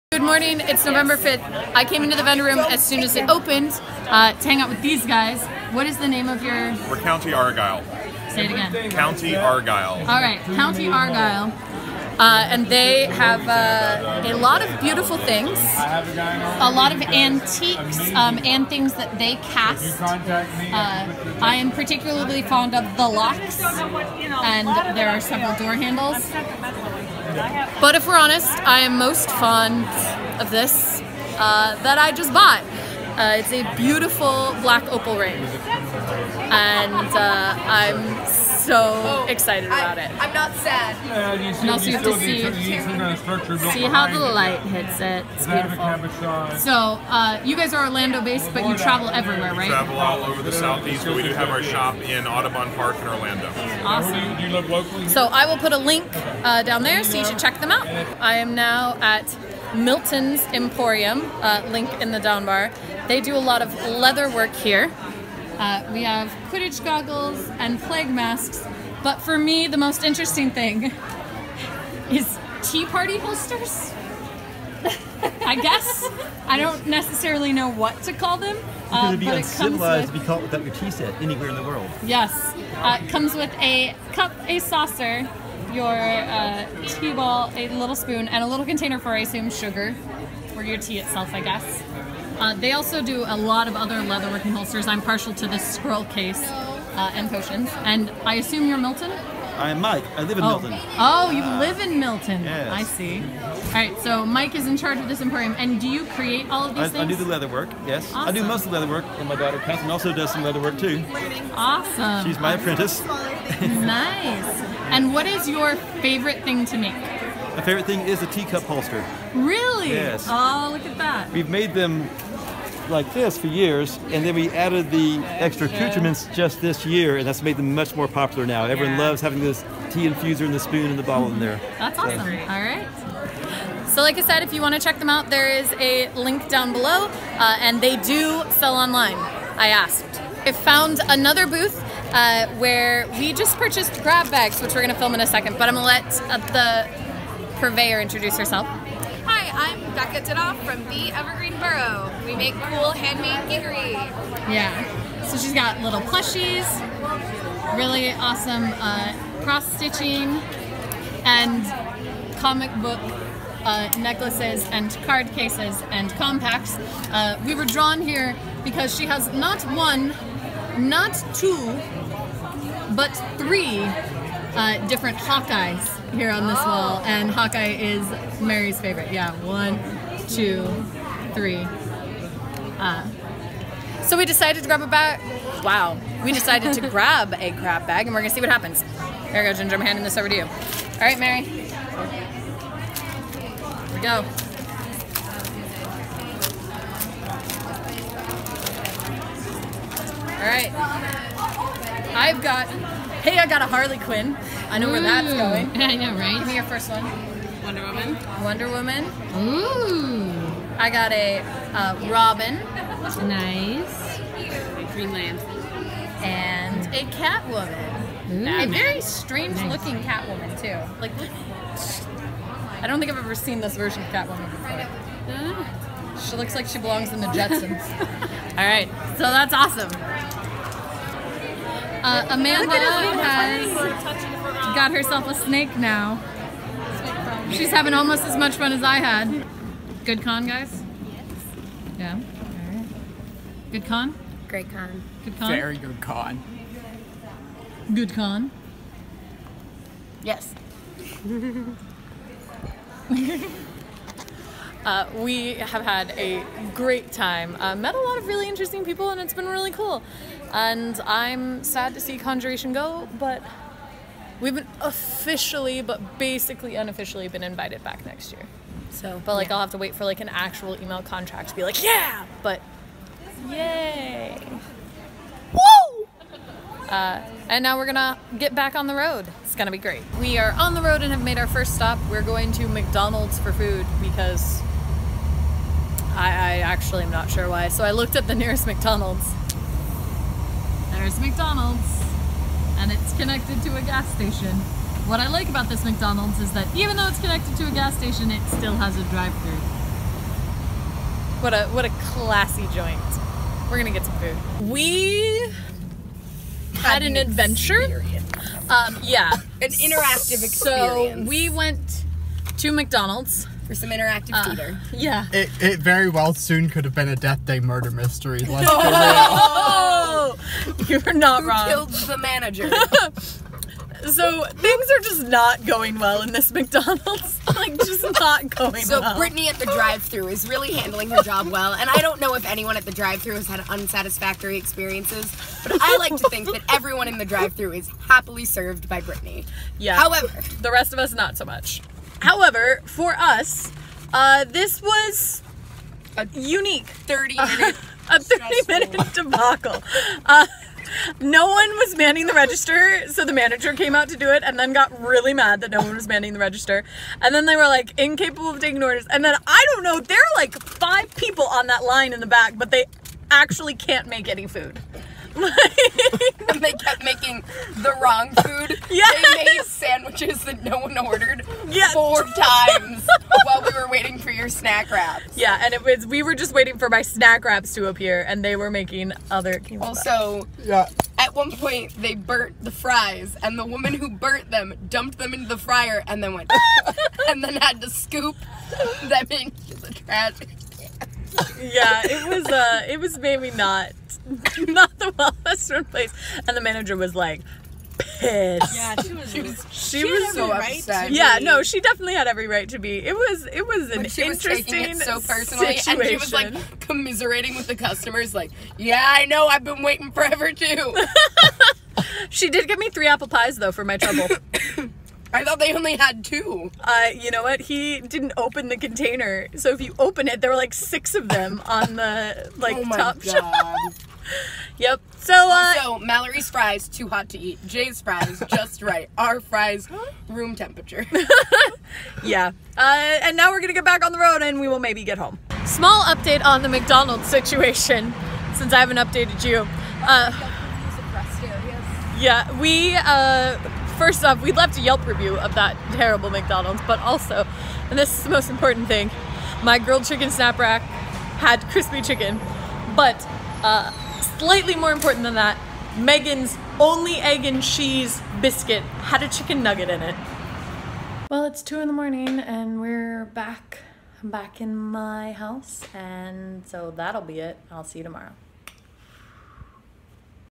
CONjuration Vlog - McDonald’s Adventure
Check out these awesome vendors and do NOT check out that McDonald’s

County Argyle (they made my lovely new ring)

Milton’s Emporium (creators of the tea party holster)

The Evergreen Burrow theevergreenburrow.etsy.com (comic pendants and grab bags that I liked WAY too much)